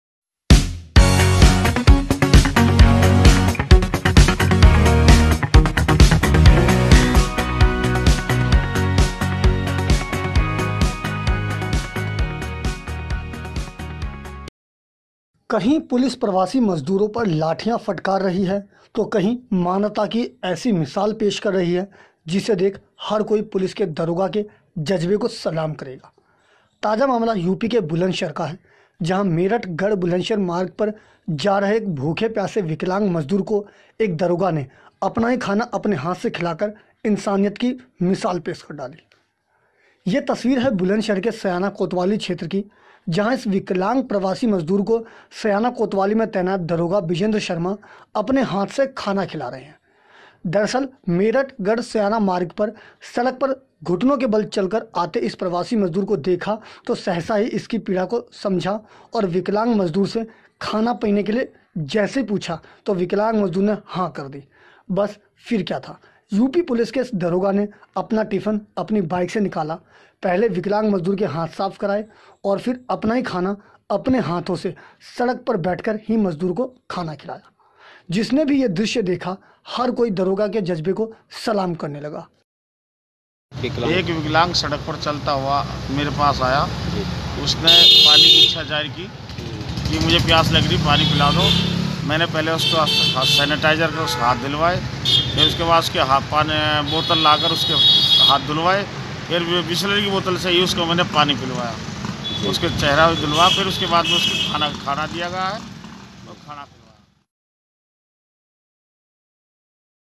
बुलंदशहर में UP Police के दरोगा के जज्बे को सलाम | Bulandshahr | Hamwatan TV
कही पुलिस प्रवासी मजदूरो पर लाठियां फटकार रही है तो कही मानवता की ऐसी मिसाल पेश कर रही है जिसे देख हर कोई  पुलिस के दरोगा के जज्बे को सलाम करेगा। ताजा मामला यूपी के बुलंदशहर का है, जहाँ मेरठ- गढ़- बुलंदशहर मार्ग पर जा रहे एक भूखे प्यासे विकलांग मजदूर को एक दरोगा ने अपना ही खाना अपने हाथ से ख़िलाकर इंसानियत की मिशाल पेश कर डाली।
ये तस्वीरें है बुलंदशहर के स्याना कोतवाली क्षेत्र की,  जहाँ इस विकलांग प्रवासी मजदूर को स्याना कोतवाली में तैनात  दरोगा बिजेंद्र शर्मा अपने हाथ से खाना खिला रहे है।
दरअसल आज मेरठ- गढ़- स्याना मार्ग पर सड़क पर घुटनों के बल चलकर आते इस प्रवासी मजदूर को देखा तो सहसा ही इसकी पीडा को समझा और विकलांग मजदूर से खाना- पानी के लिये जैसे ही पूछा  तो विकलांग मजदूर ने हा कर दी, बस फिर क्या था। यूपी पुलिस के इस दरोगा ने अपना टिफन अपनी बाइक से निकाला, पहले विकलांग मजदूर के हाथ साफ कराये और फिर अपना ही खाना, अपने हाथ से सड़क पर बैठकर ही मज़दूर को खाना खिलाया। जिसने भी ये दृश्य देखा हर कोई दरोगा के जज्बे को सलाम करने लगा।